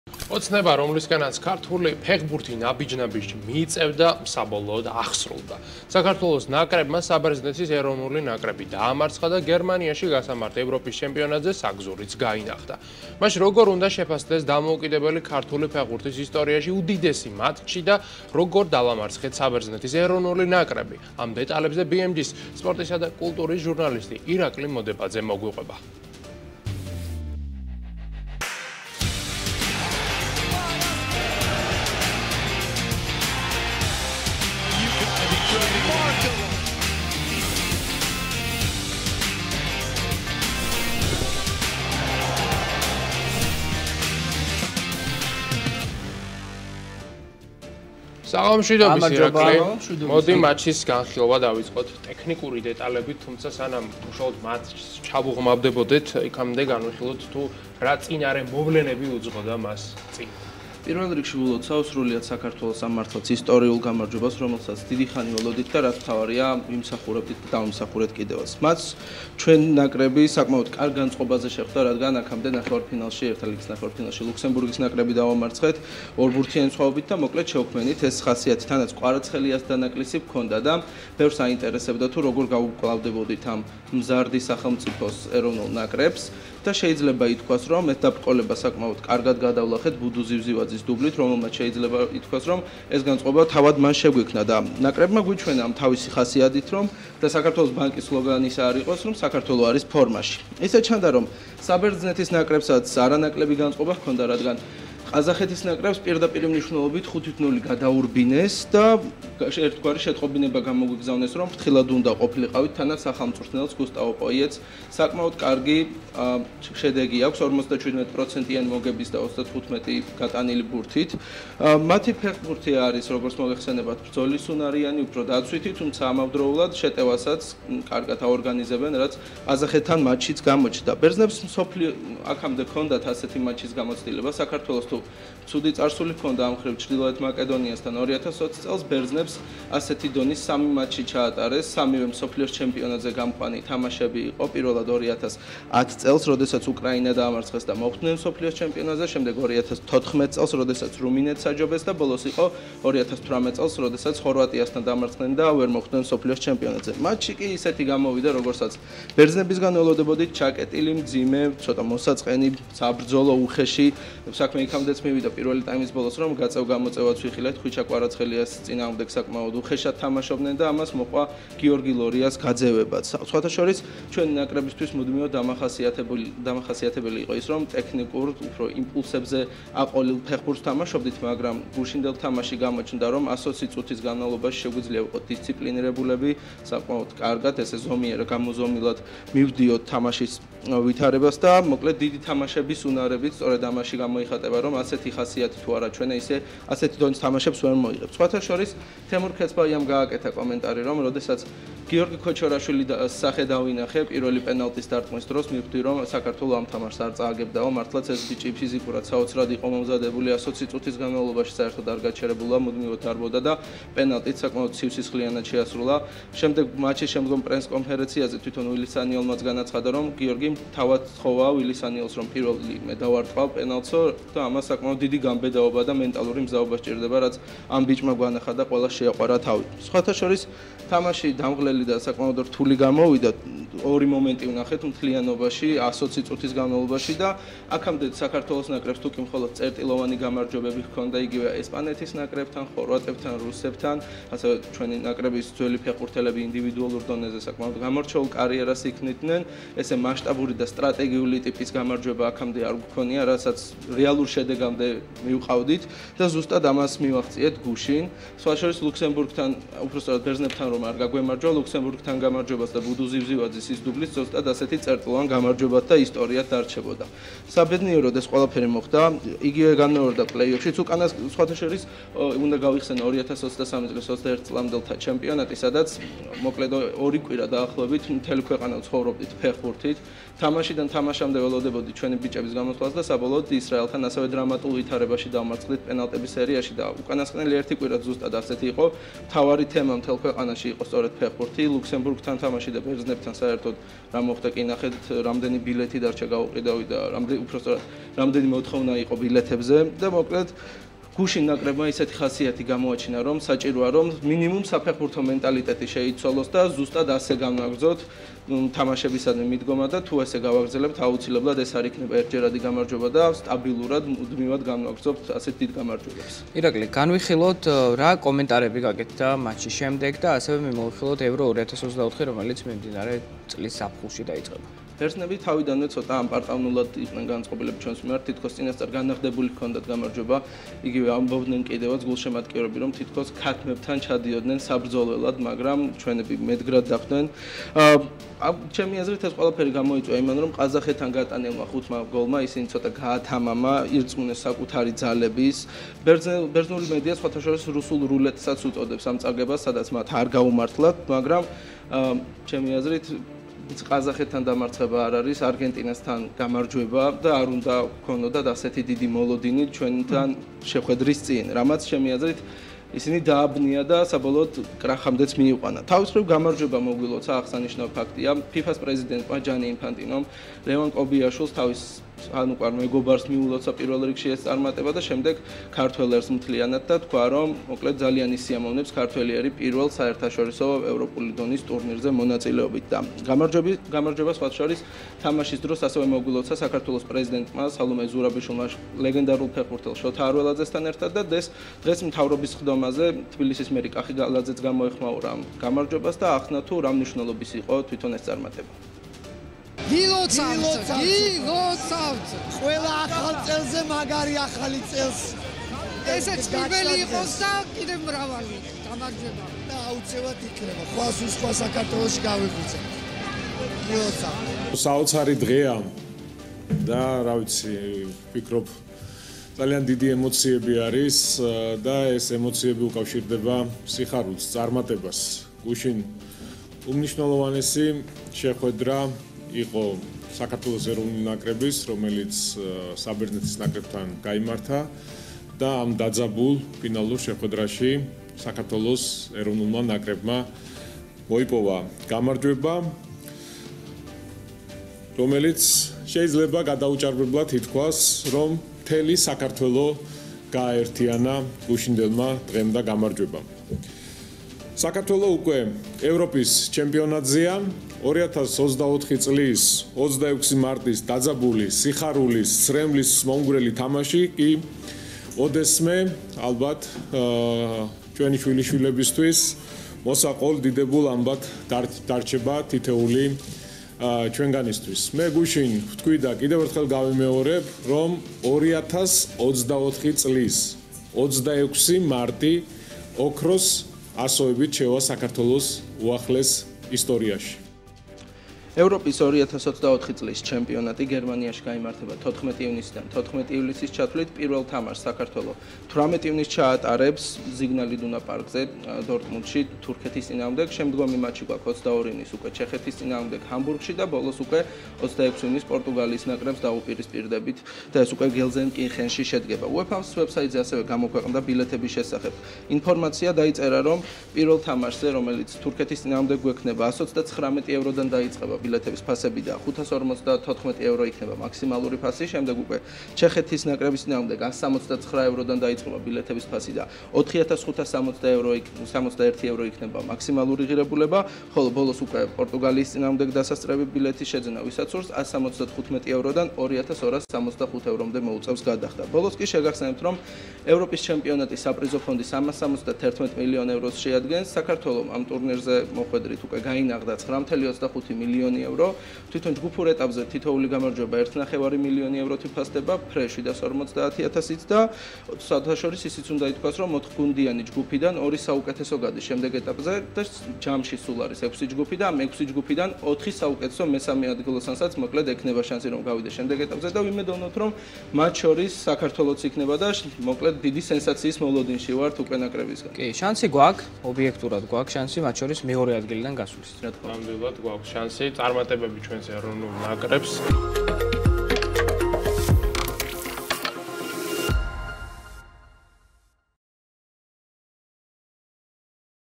ისტორია დაიწერა - საქართველო ევროპის ჩემპიონატზე იასპარეზებს;
ირაკლი მოდებაძე - BMG-ის რეპორტიორი;